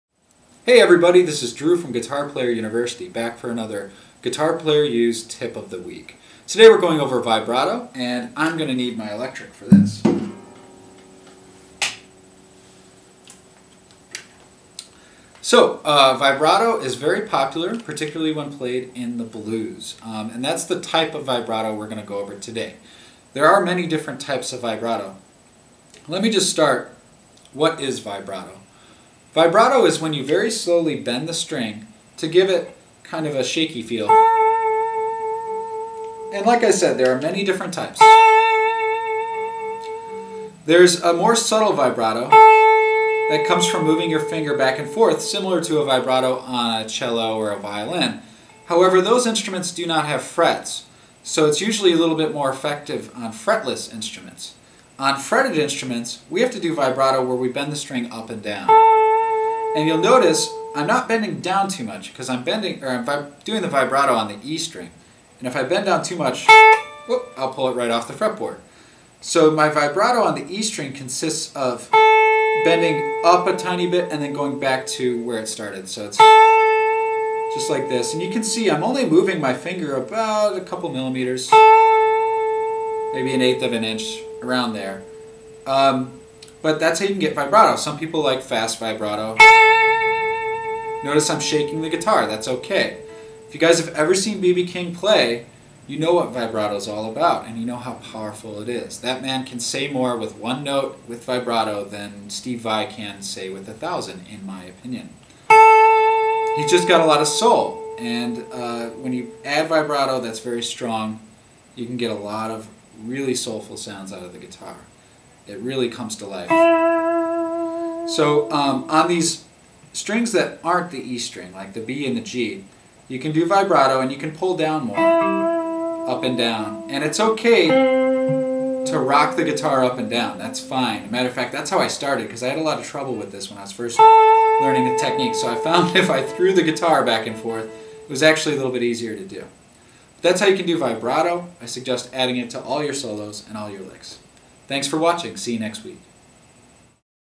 Guitar Lesson Vibrato
Free guitar lessons - This weeks free guitar lesson goes over vibrato, what it is and how to play vibrato to get better sounds from your guitar. For more free guitar lessons please visit GuitarPlayerU.com